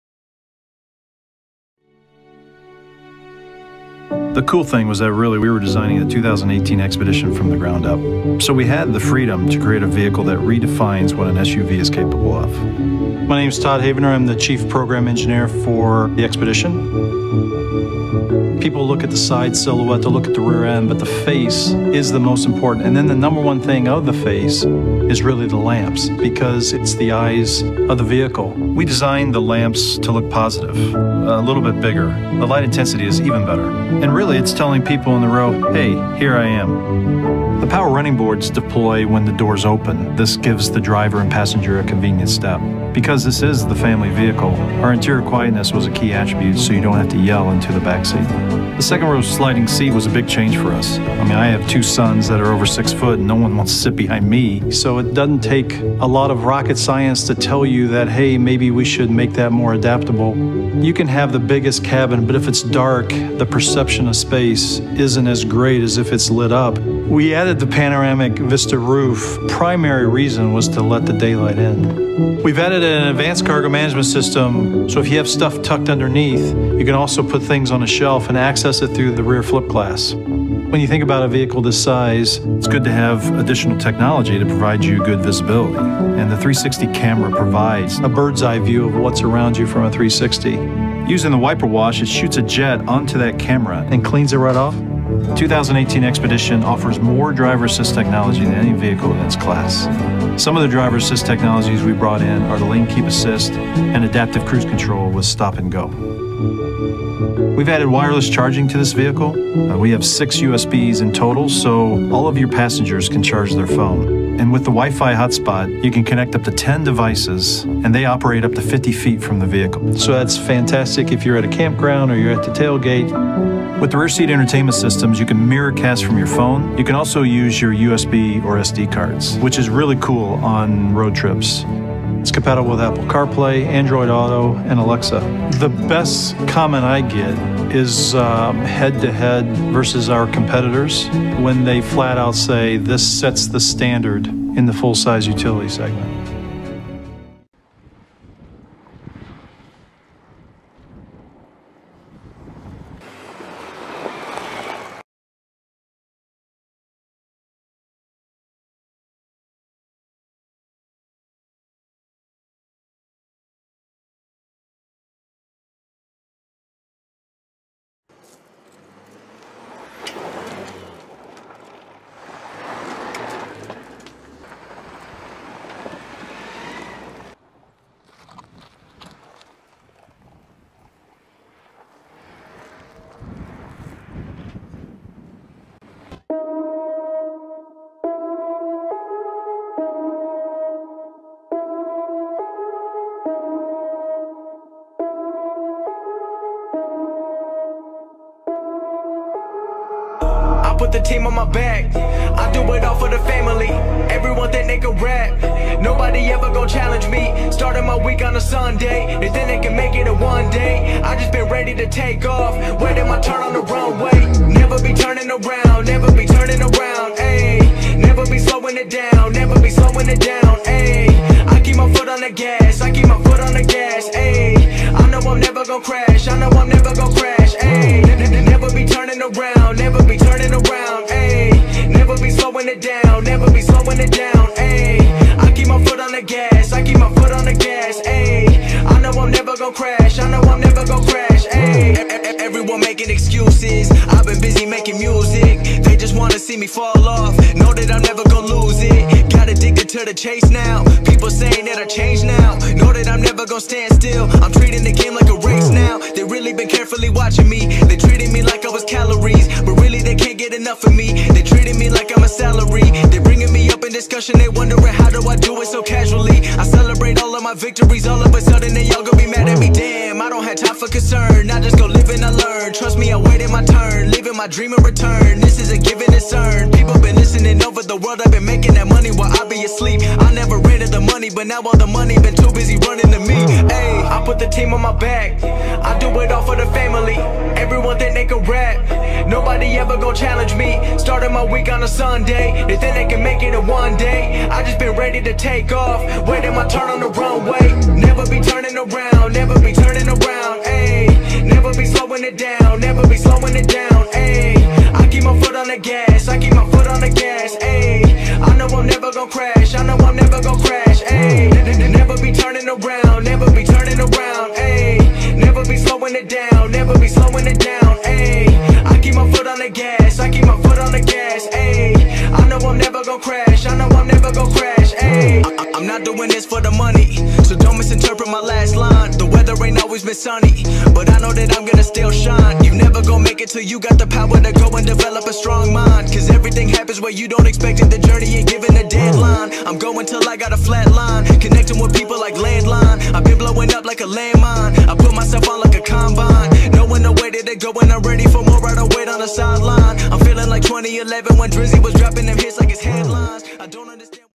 2018 Ford Expedition - FULL REVIEW, Interior & Exterior | Auto Technology
2018 Ford Expedition - FULL REVIEW, Interior & Exterior | Auto Technology

The 2018 Ford Expedition has the towing abilities and ruggedness buyers expect from a full-size SUV but hiding beneath its hulking shape is an impeccably usable interior, helpful technology, a powerful EcoBoost V6 engine, and expanded use of lightweight aluminum. The 2018 Expedition is undoubtedly the best version yet but does Ford’s full-size SUV